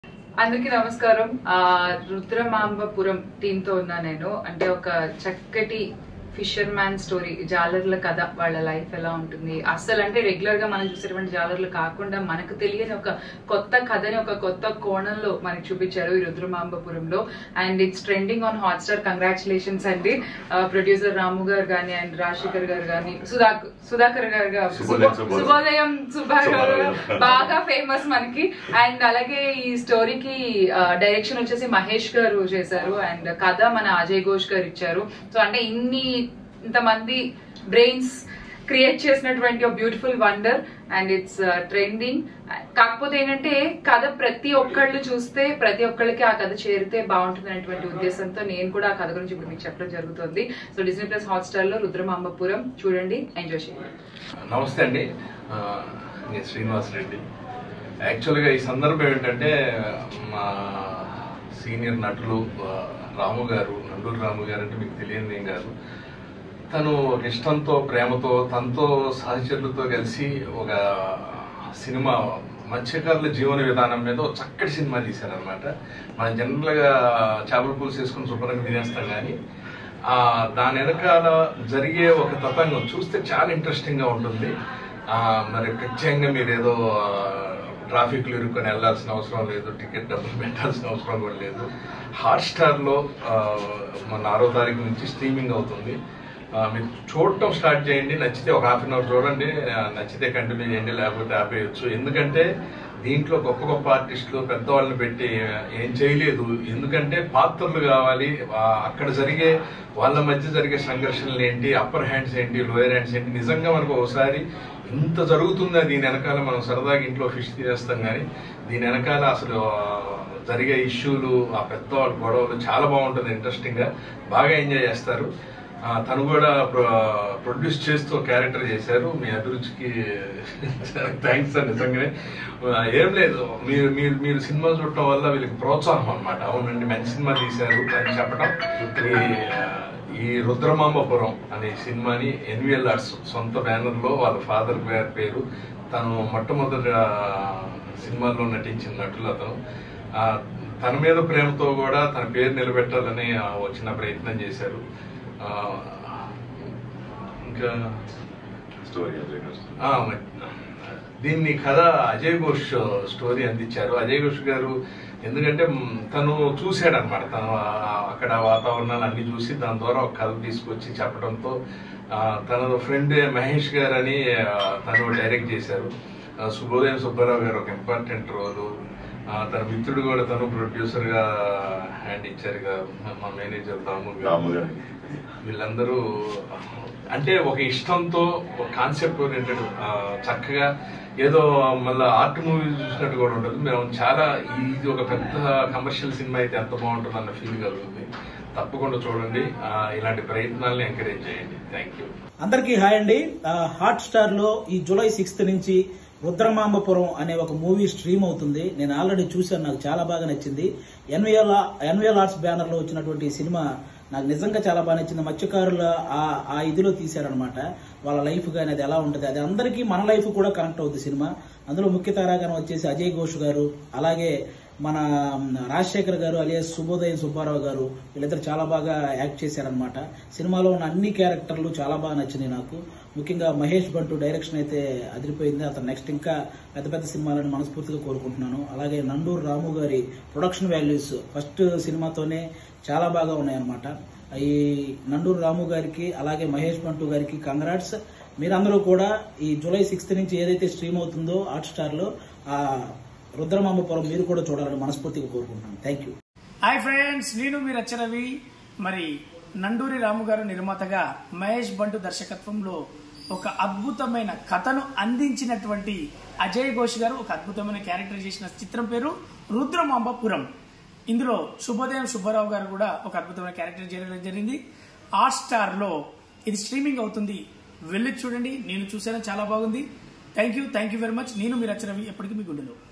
Hyper Aadi, Anchor Shyamala, Srinivas Reddy Bytes About Rudramambapuram | MS Talkies
Watch HyperAadi, Anchor Shyamala, SrinivasReddy Bytes About Rudramambapuram Team